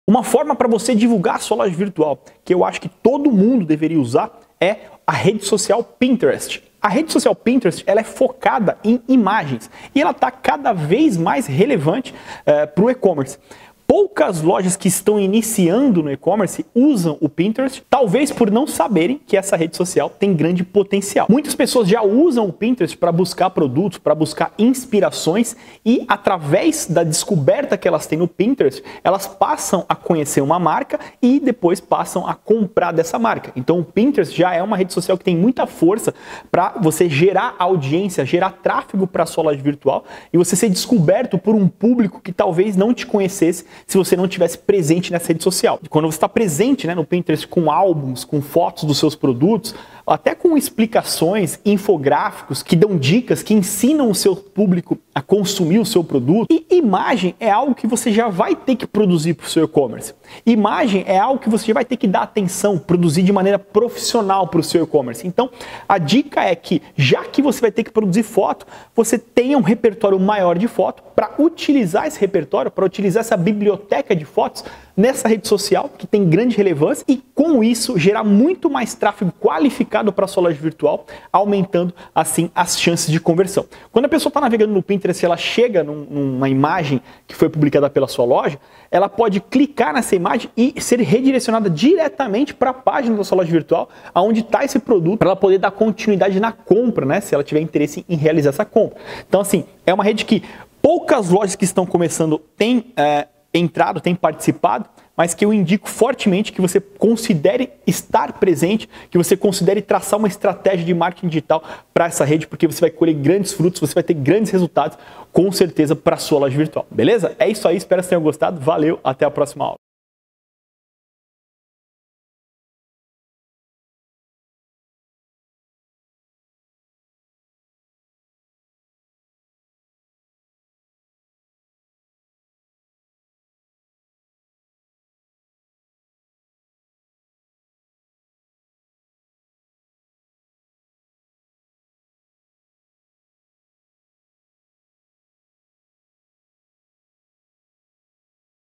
Porque você AINDA NÃO USA isso para divulgar sua Loja Virtual?
Nessa aula Rafael Souza fala de uma forma para divulgar sua loja virtual muito eficiente, porém que poucas lojas virtuais utilizam. Assista!